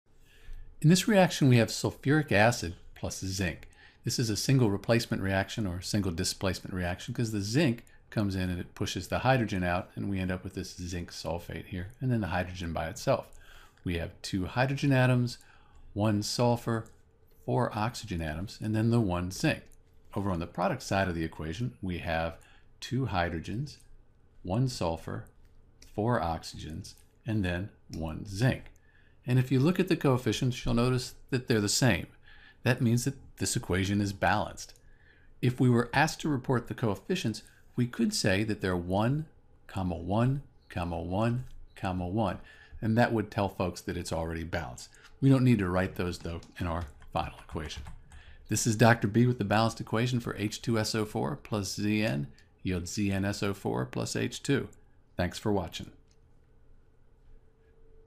How to Balance H2SO4 + Zn = ZnSO4 + H2 (Sulfuric acid + Zinc)
In this video we will balance the equation H2SO4 + Zn = ZnSO4 + H2 . for video guides on balancing equations and more!

To balance H2SO4 + Zn = ZnSO4 + H2 you will need to be sure to count all of atoms on each side of the chemical equation.

Once you know how many of each type of atom you can only change the coefficients (the numbers in front of atoms or compounds) to balance the equation for Sulfuric acid + Zinc .